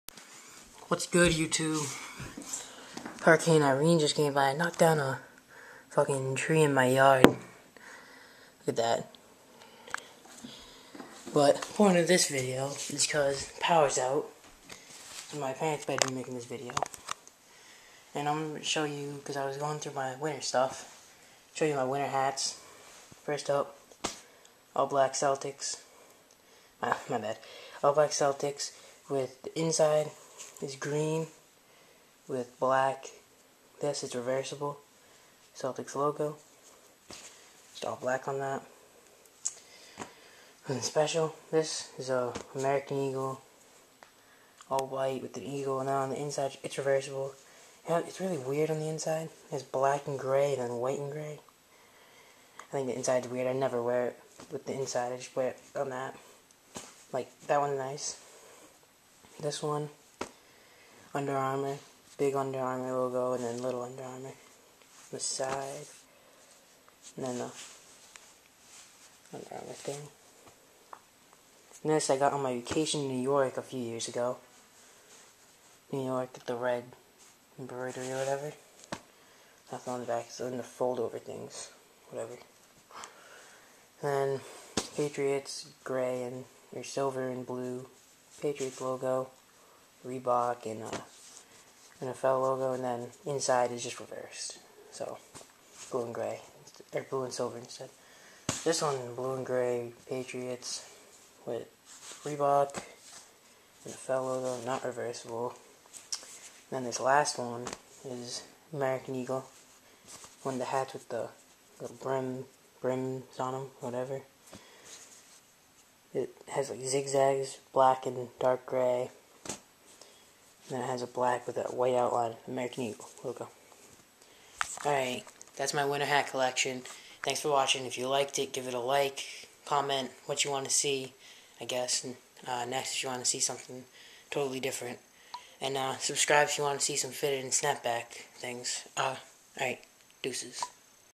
My Winter Hat/Beanie Collection
This is a really small collection but i thought i would make a vid thanks for watchin if u liked it give it a thumbs up and sub for some fitted and snapback vids